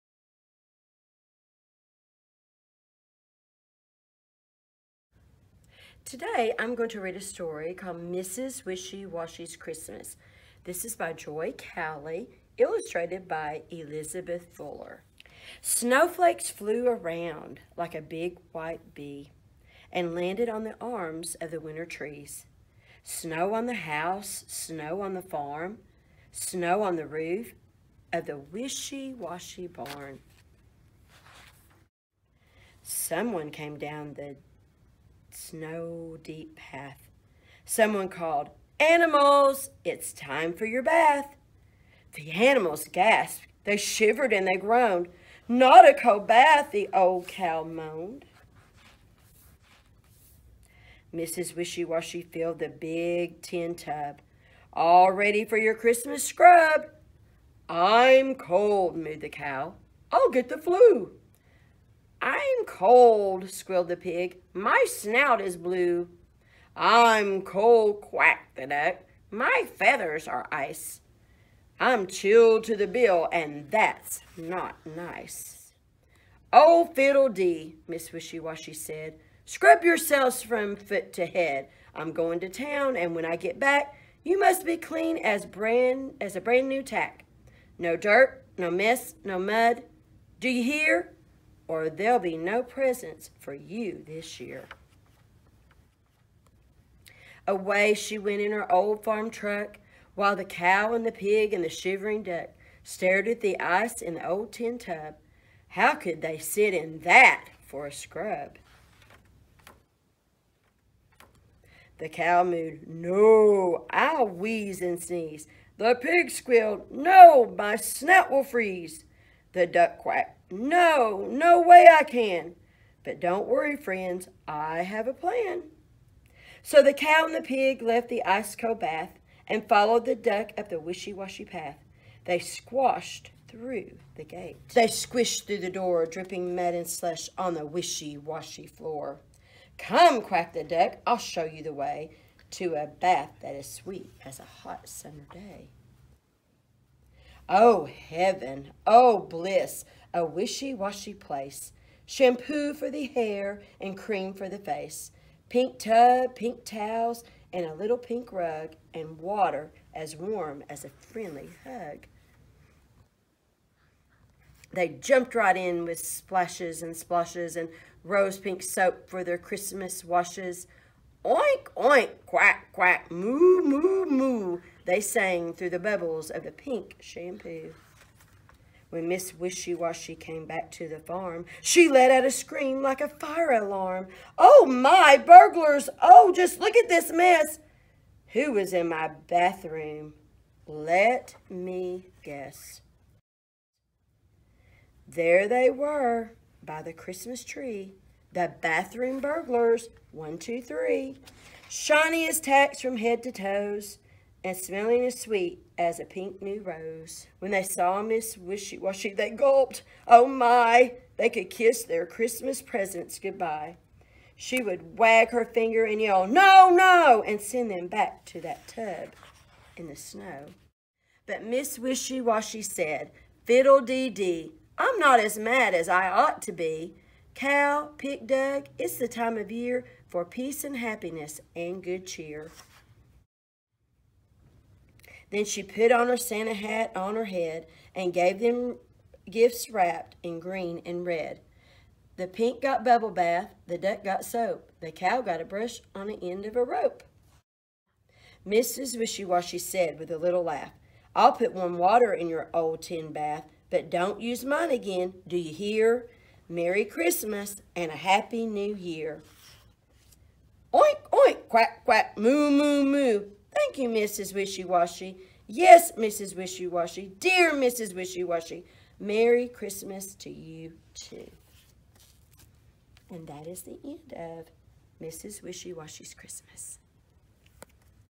Sentell Reading Mrs Wishy Washy's Christmas
Listen and watch as Mrs. Sentell shares a Christmas Story